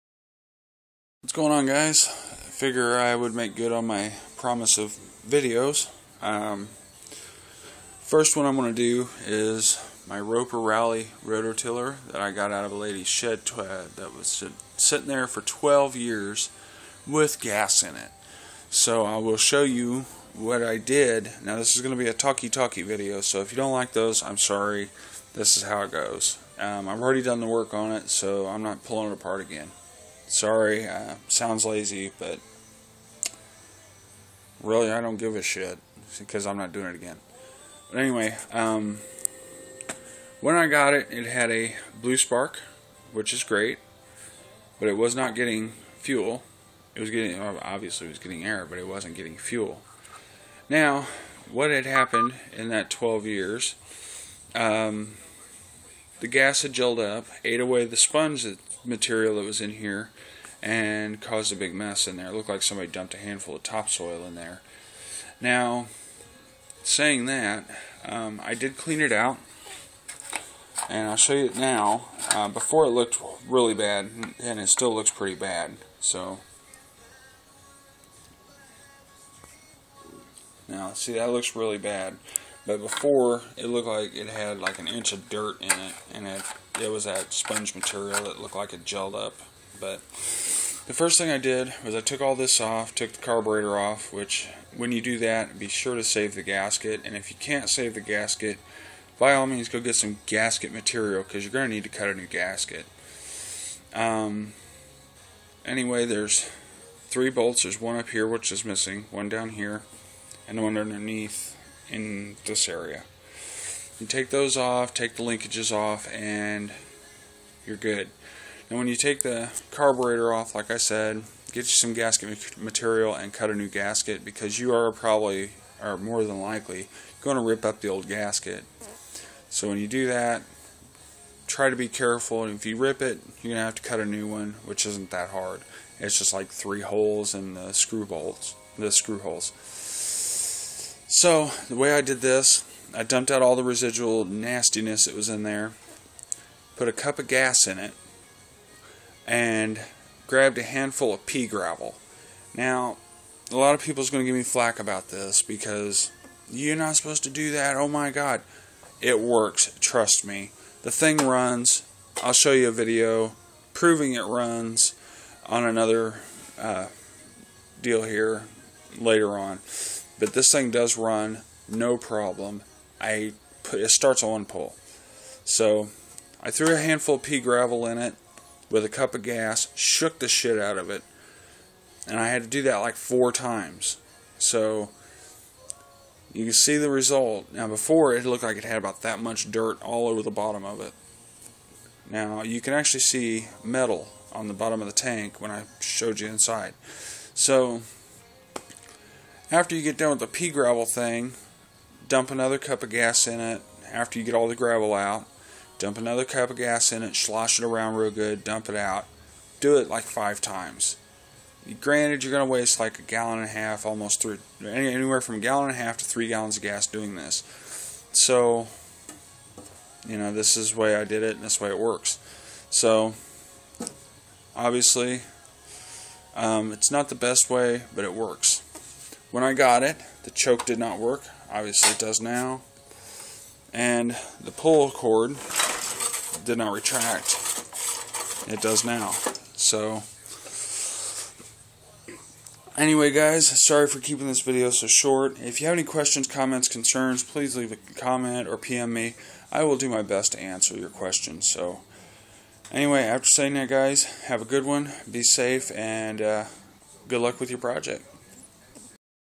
Describing the repairs on a 83 roper rally tiller
Just a descriptive video on how I repaired my roper rotor tiller.